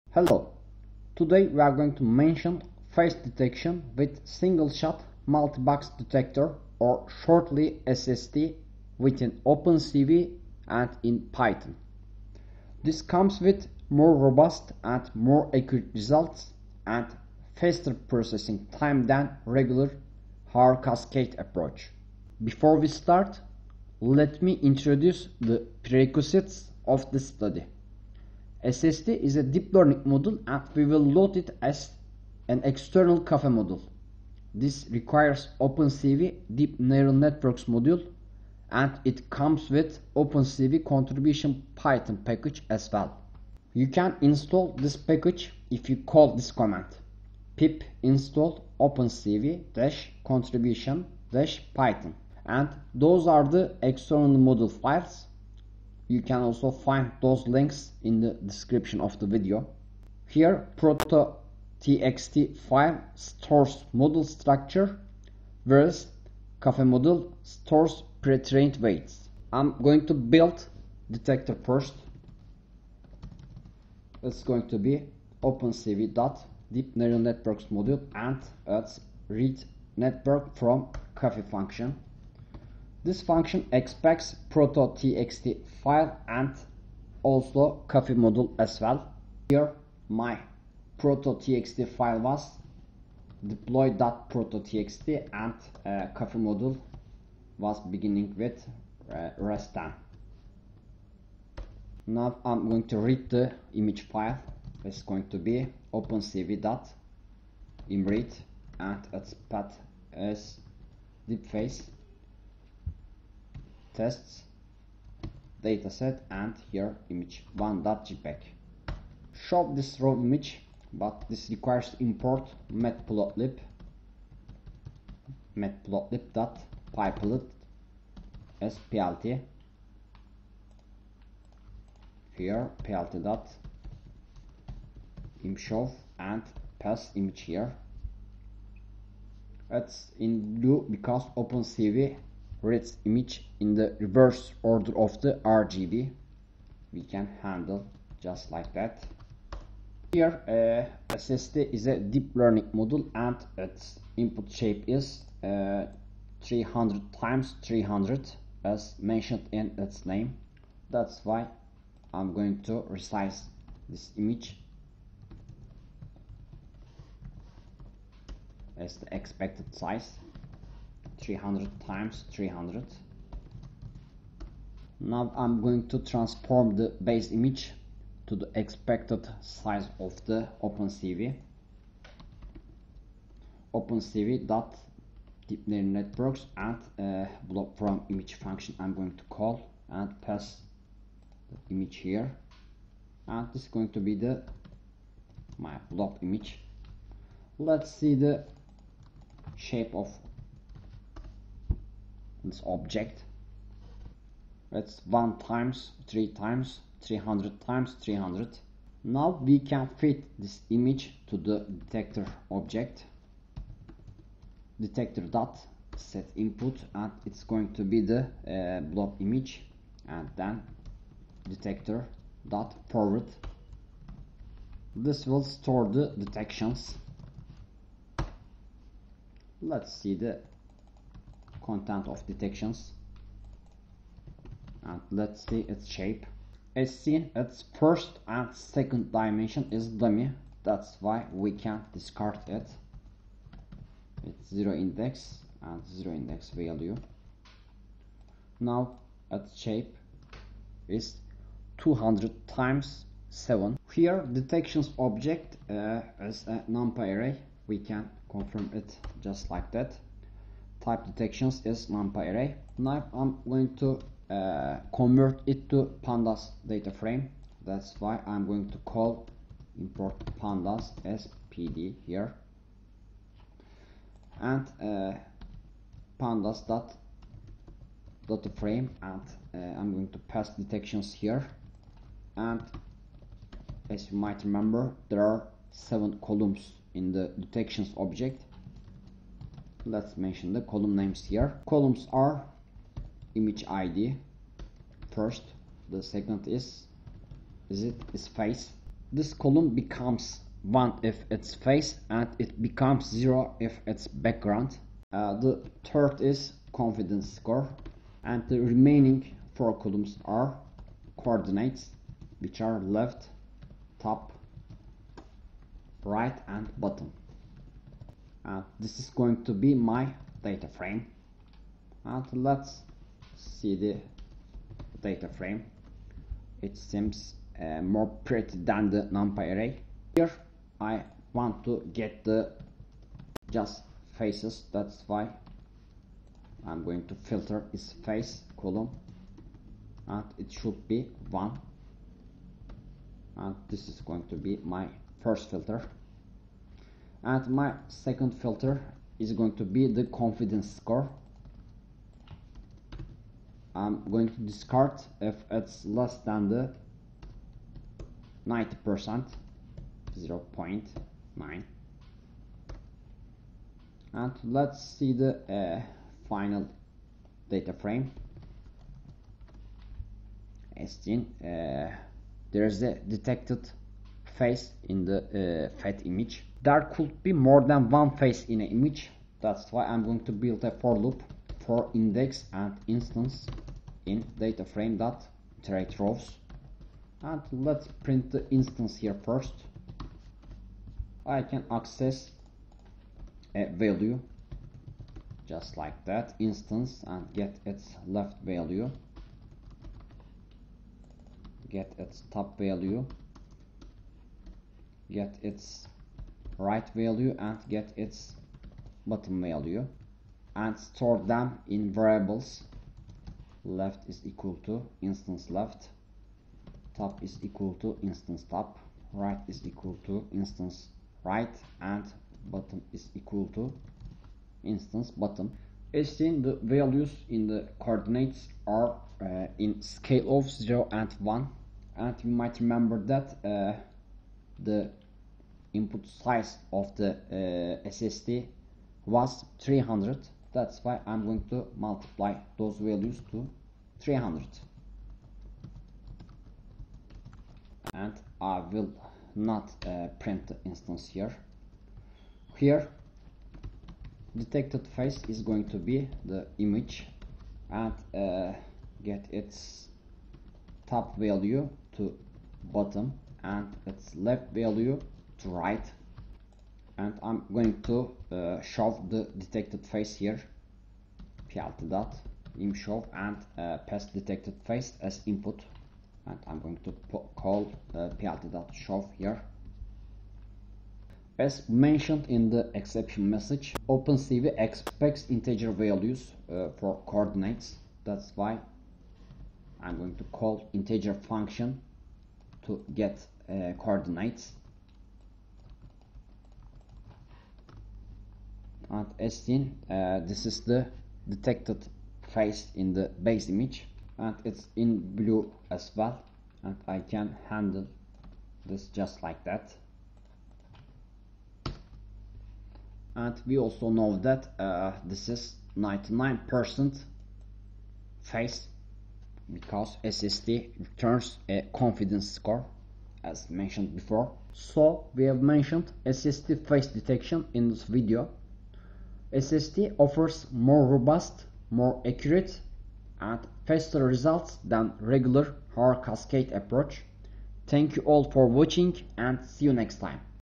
Deep Face Detection with OpenCV in Python: SSD
Face detection is an early stage of a face recognition pipeline. Today, we are going to mention single shot multibox detector or shortly SSD for face detection. It is a deep learning based technique and it comes with more robust, more accurate and faster results than regular haar cascade. Besides, it is the fastest one among haar, mtcnn and dlib hog.

If you do like my videos, you can support my effort with your financial contributions on